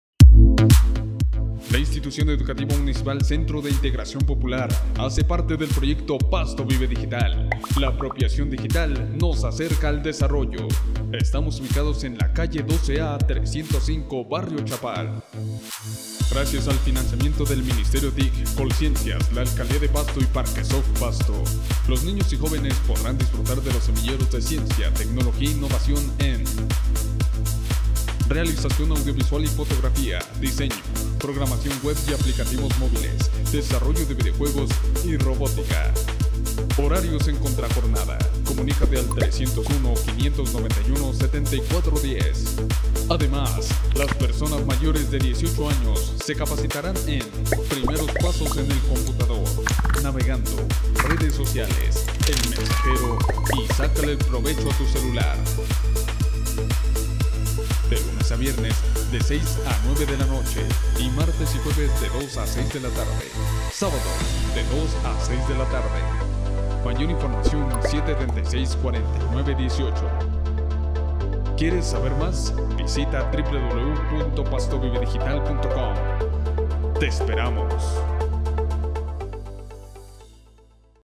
Las TIC aportan a la educación. I.E.M Centro de Integración Popular
Como parte del proyecto Pasto Vive Digital, en su entregable Red de Centros, la institución educativa Centro de Integración Popular participa activamente de procesos que le aportan al desarrollo a través de las TIC